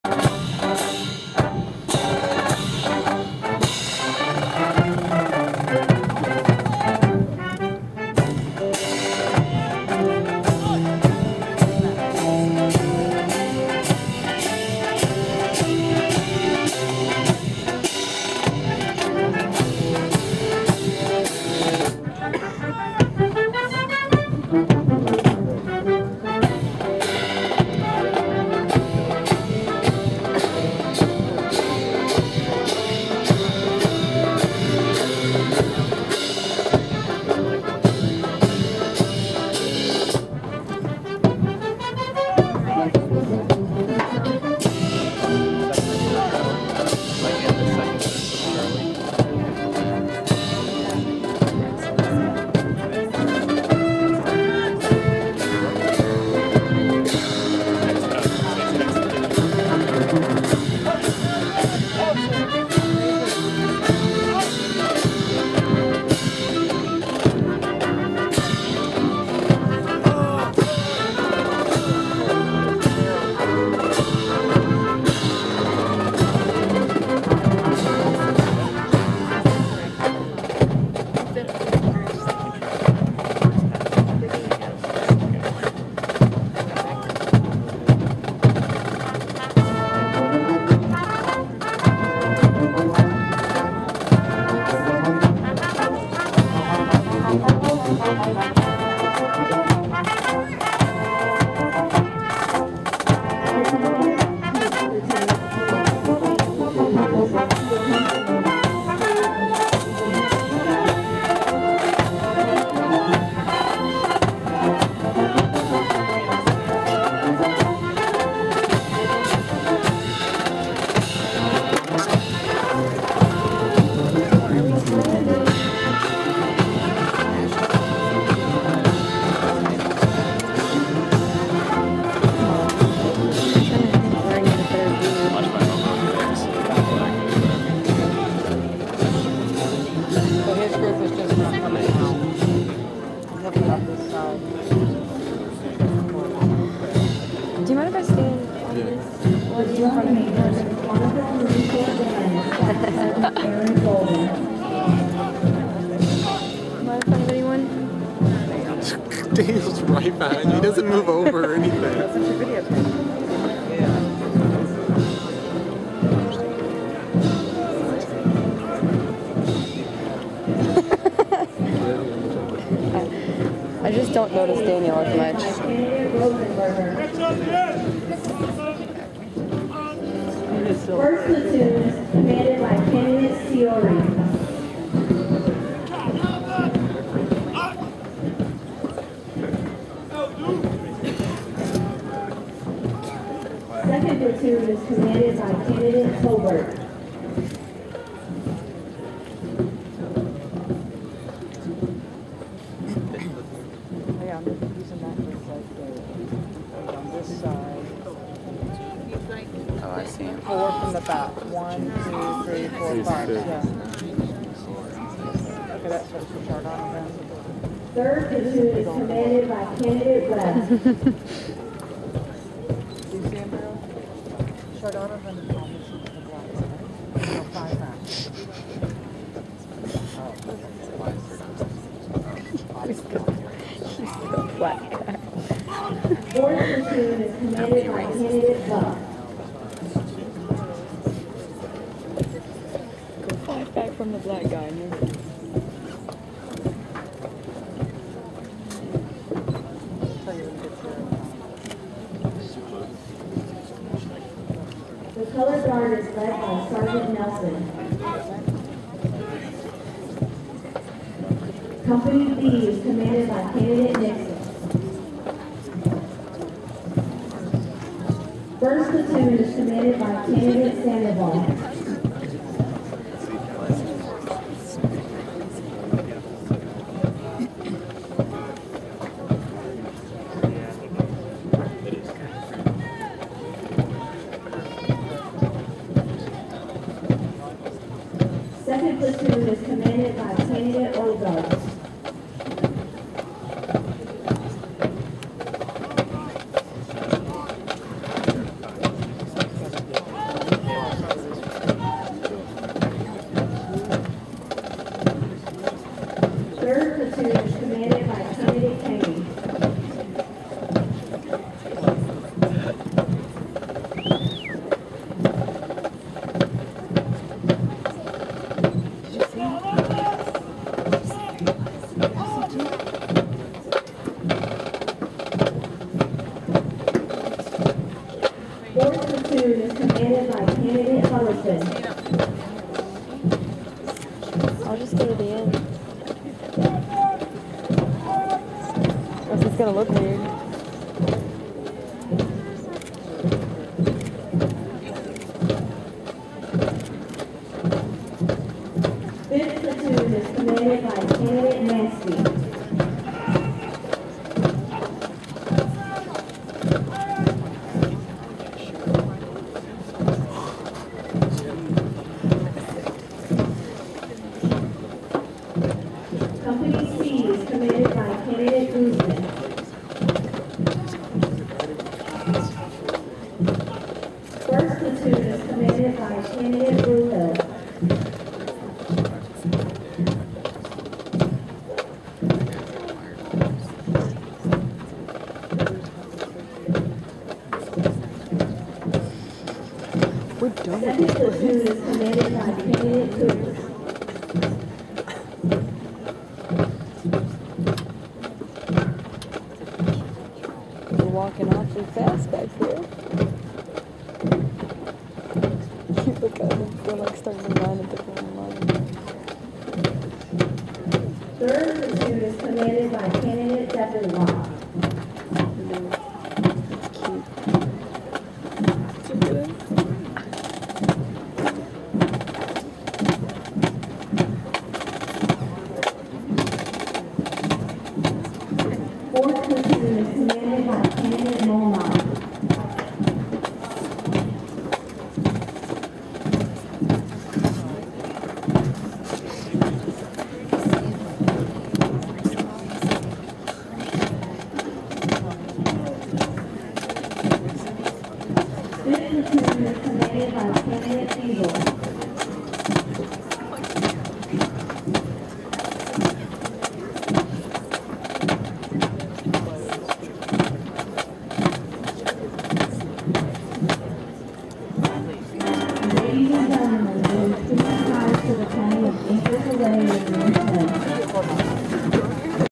Marine Corps Officer Candidates Graduate
Candidates executing "pass in review" at the end of our graduation.